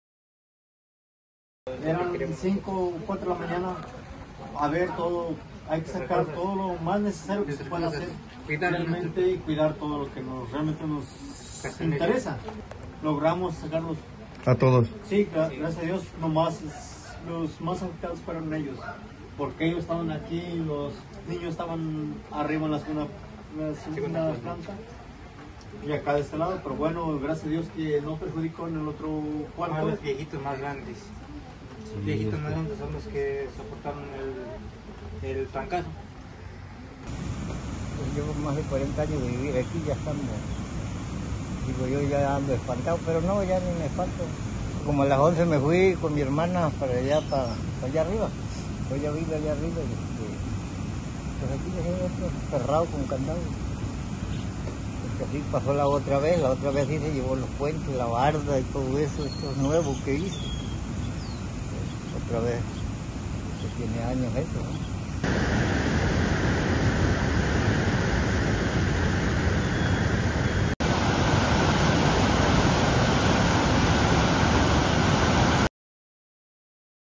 Más de mil 500 casas dañadas por paso de ‘Chris’ en Veracruz
Más de mil 500 viviendas se vieron afectadas por el paso de la tormenta tropical Chris en al menos 25 municipios de Veracruz, indicó la Secretaría de Protección Social del estado. Info